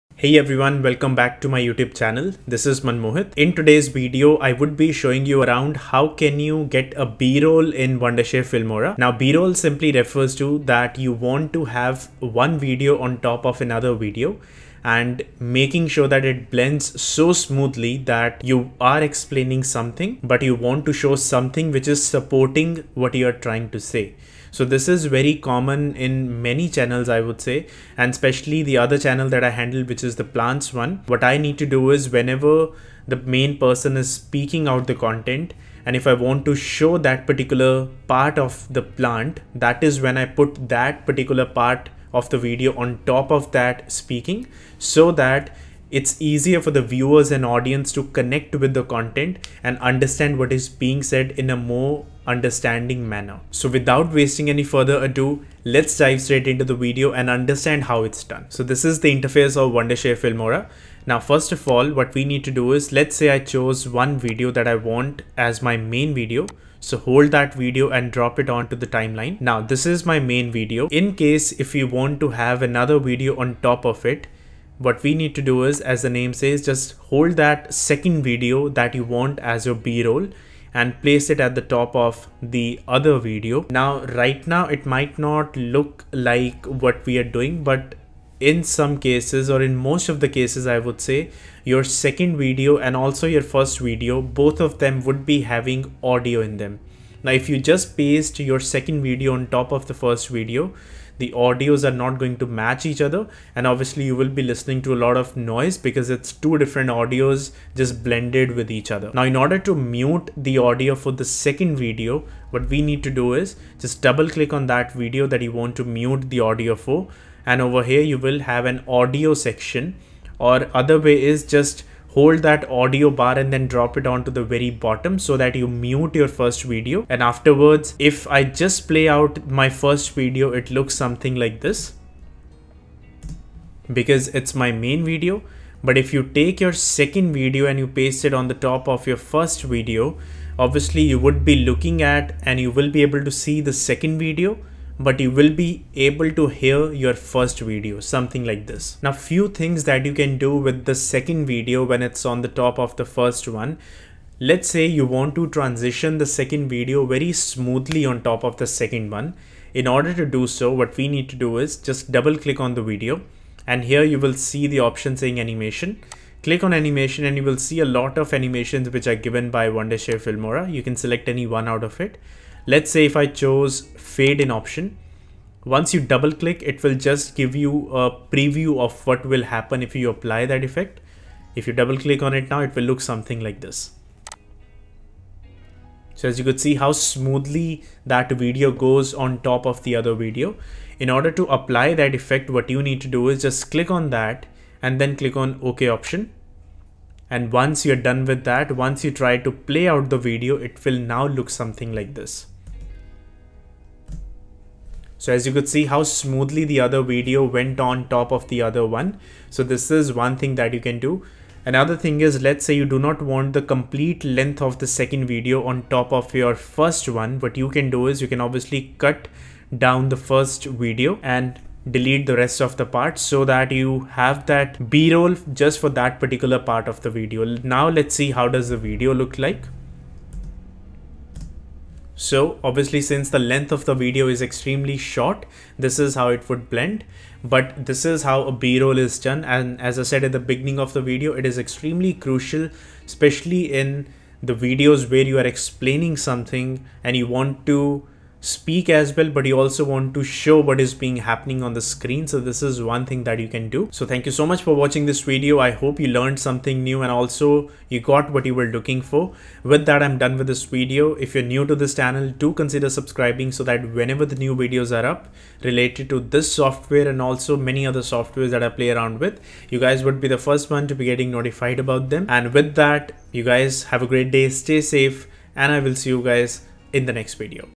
How to Add a Video on another Video in Filmora | B-Roll Tutorial
In this video, I've explained how to get a b-roll in Wondershare Filmora, or how to have a video on top of another video in Wondershare Filmora. this method will work with any version of Wondershare Filmora, so do watch the video until the very end, to understand how to get another video on top of one video in the form of a b-roll.